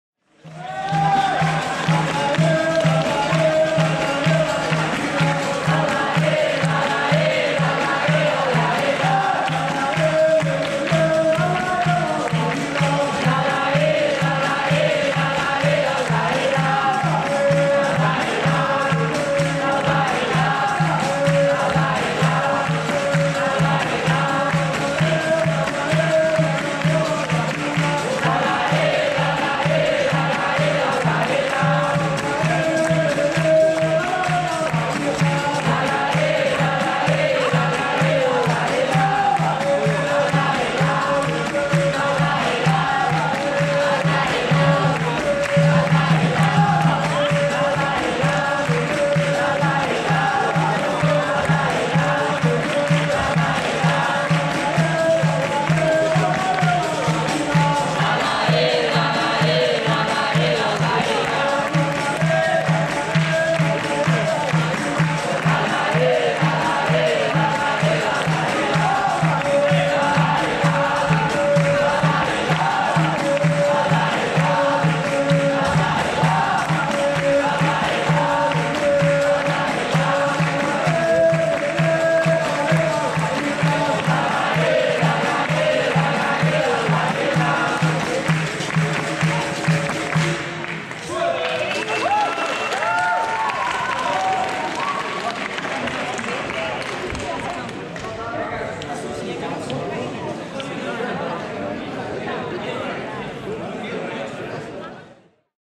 Troca de cordas: Amarela | 25° Iê Berimbau Capoeira Festival - Bélgica 2022
Grupo Internacional Mundo Capoeira
Mestre Dendê
Diest - Bélgica (Belgium / België)
2022